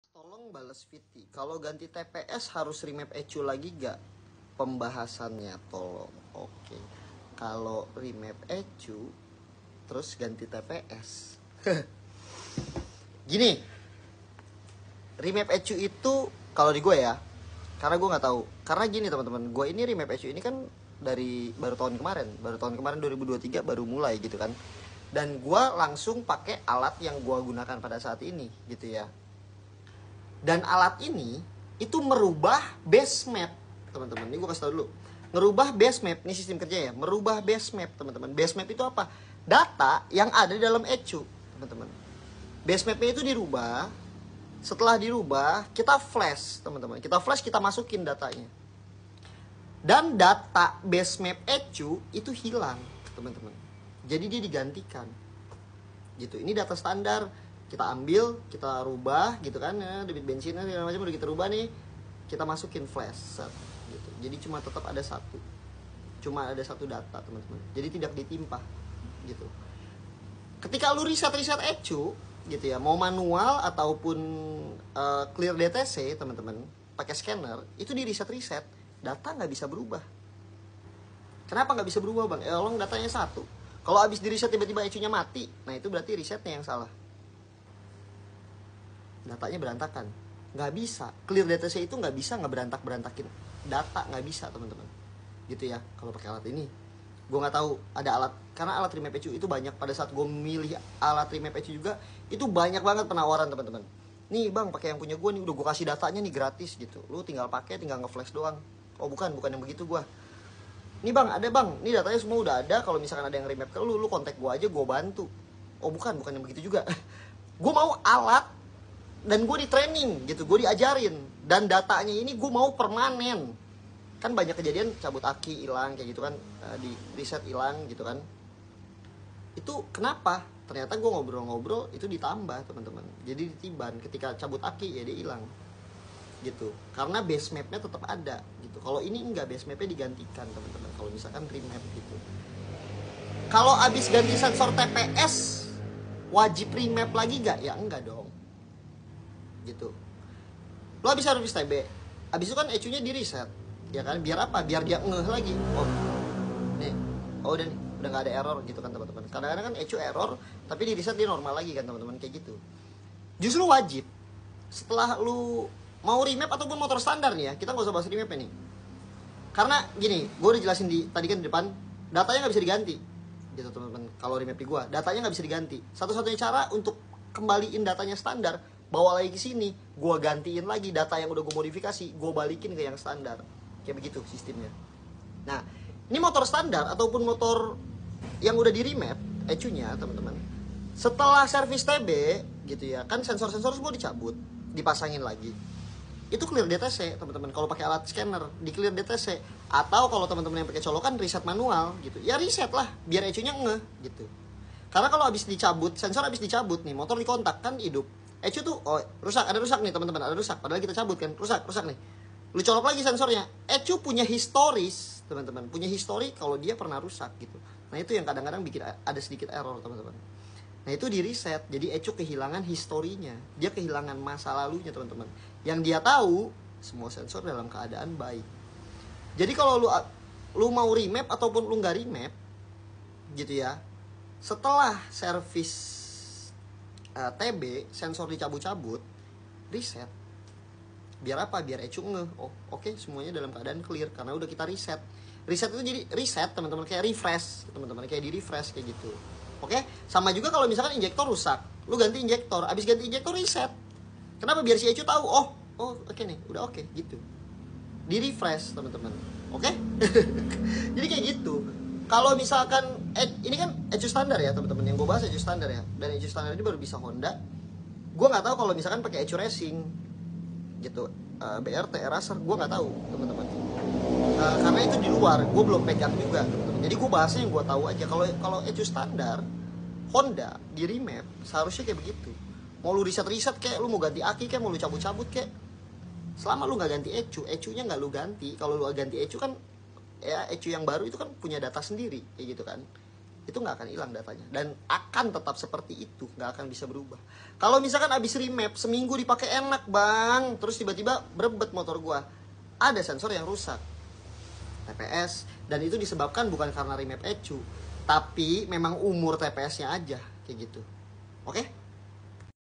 Ganti TPS Harus Remap ulang ECU??
Hallo genks jadi di video kali ini gw mau bahas tentang Sensor TPS!!.
Kalo kalian motor nya sudah di Remap ecunya! Dan mengganti sensor TPS entah racing/standart. Apakah harus remap ulang???

Simak sampai habiss!!!